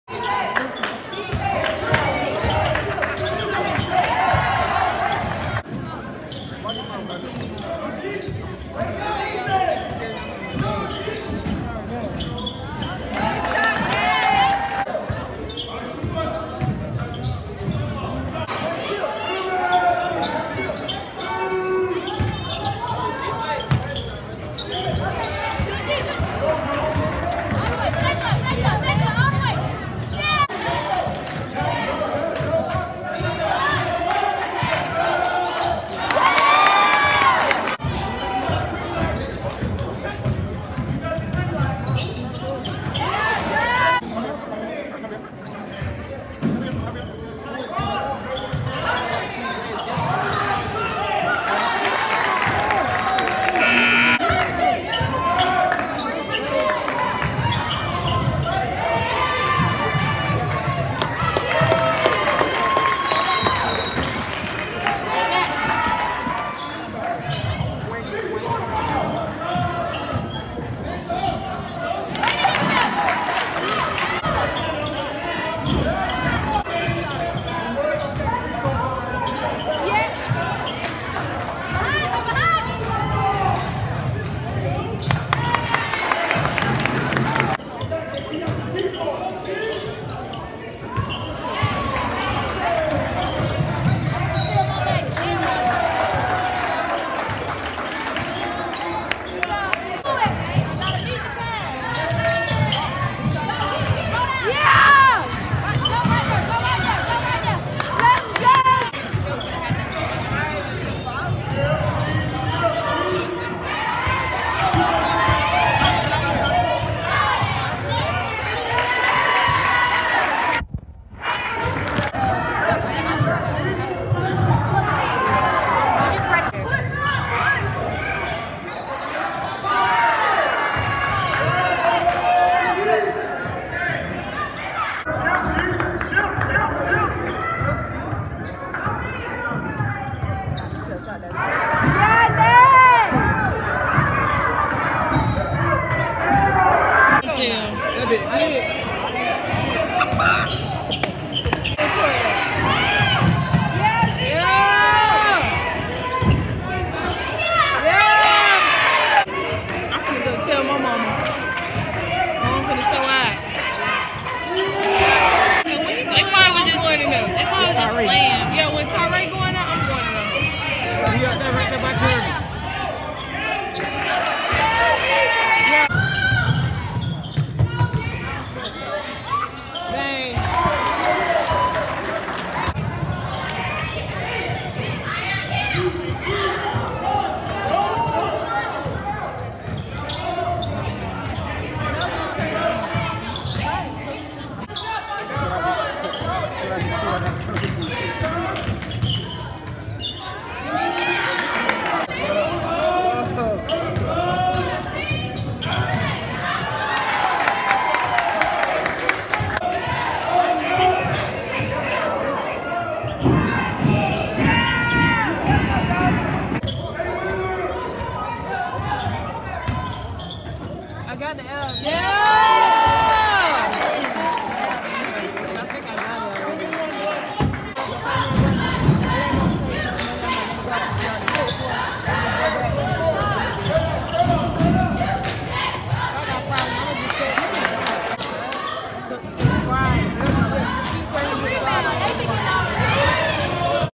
Na’Haviya Paxton Highlights v Creekside 12/09/2021 Quadruple-Double
24 points, 10 rebounds, 10 steals, 10 assists, 2 blocks